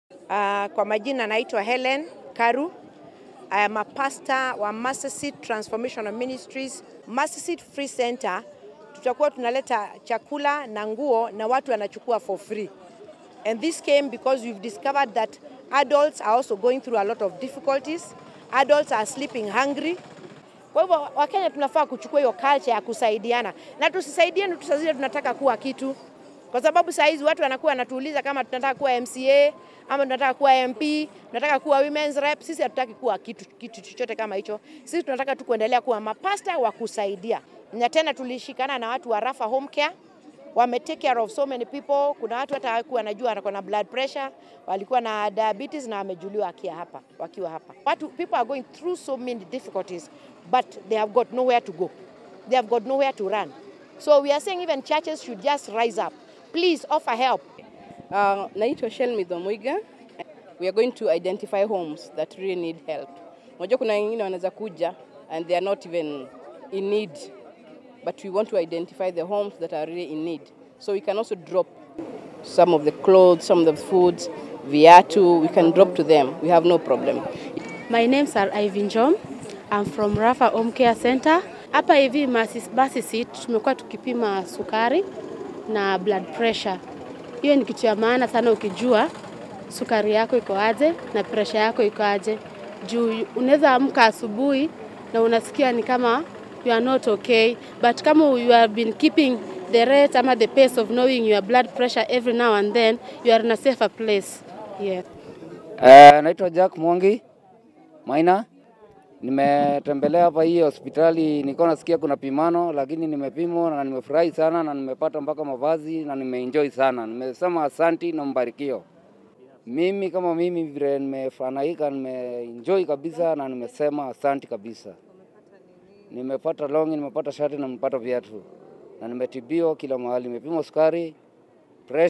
URIGITI UTARI MARIHI
AIKARI A RHODA NA KAPTEMBWA KWAMUKIRA URIGITI UTARI MARIHI NAKURU